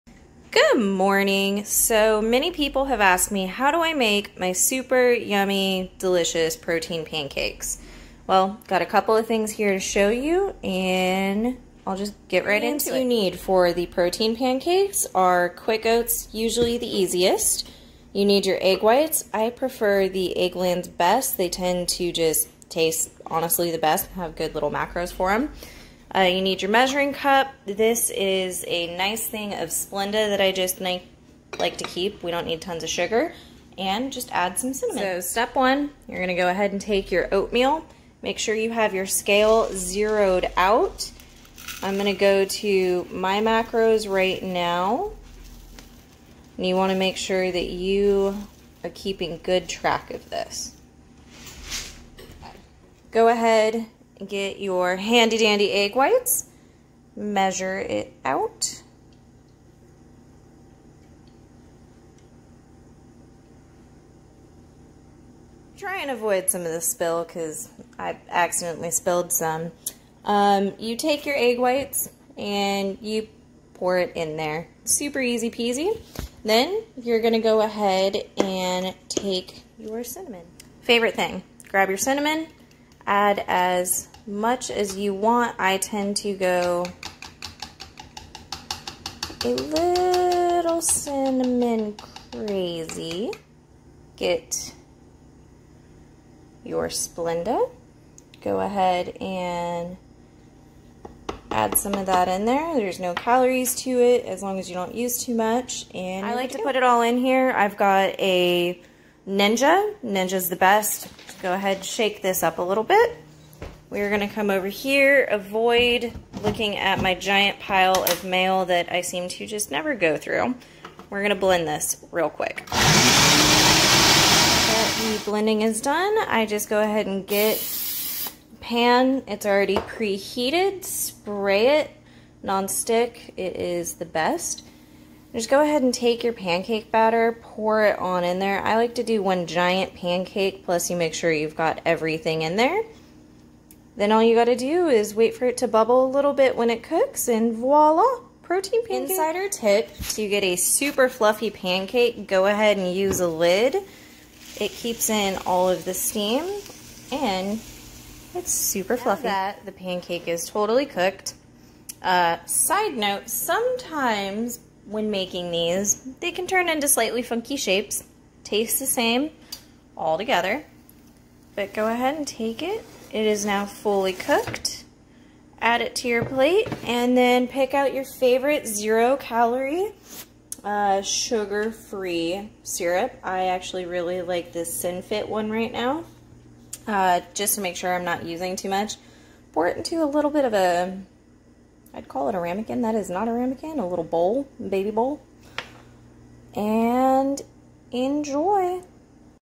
5 Steps to the Best Protein Pancakes | Bodybuilding Recipe | Healthy Eating
Learn how to make my delicious protein pancakes with these 5 easy steps! 🥞
These are packed full of protein making them perfect for weight loss, building muscle, or just a healthy lifestyle. 💪🏻

⚠️Video full of cheesey 1 liners and funny bloopers.